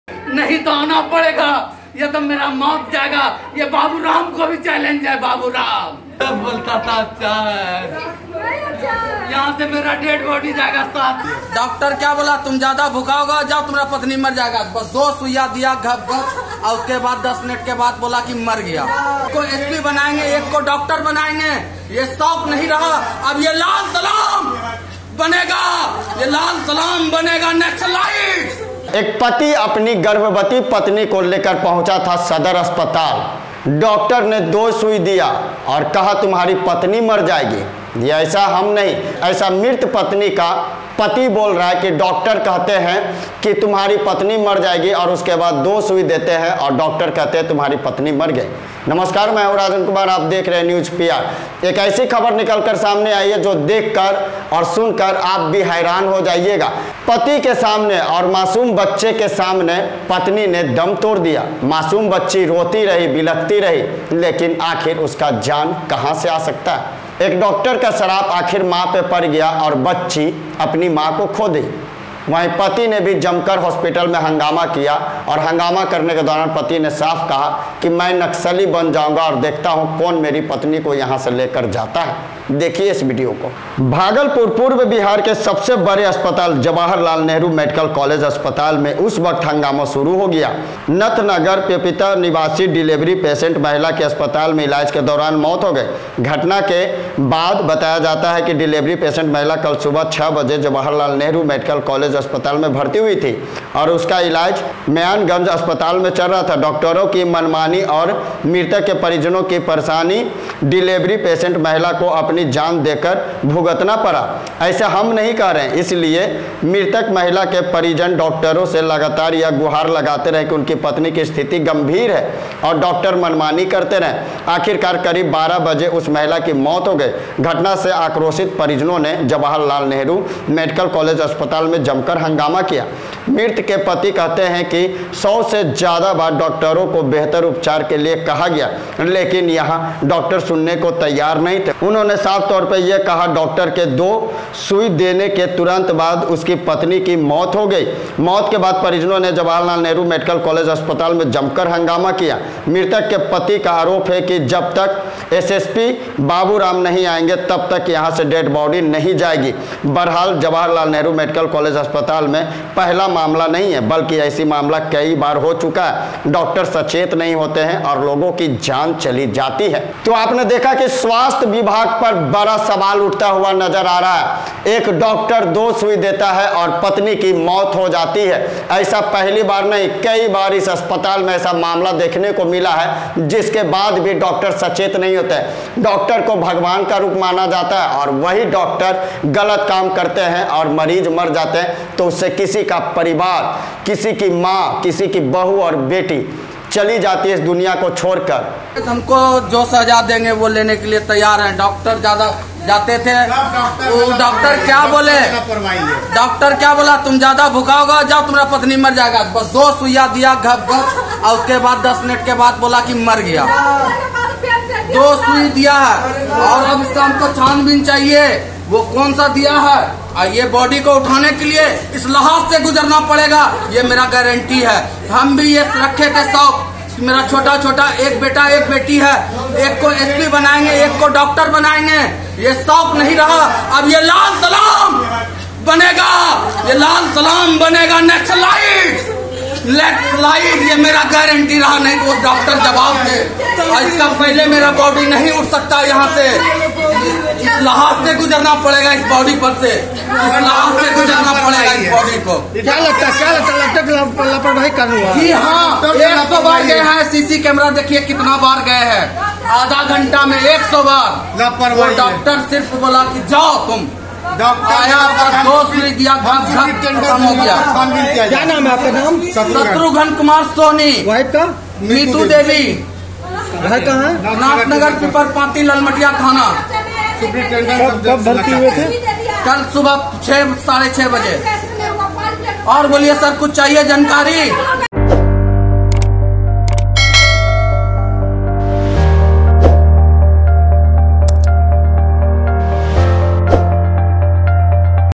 डॉक्टर साहब चलिए ना मेरी पत्नी का हालत नाजुक हैं, डॉक्टर ने कहा तुम्हारी पत्नी मर जाएगी |NewsPR
डॉक्टर साहब चलिए ना मेरी पत्नी का हालत नाजुक हैं, डॉक्टर ने कहा तुम्हारी पत्नी मर जाएगी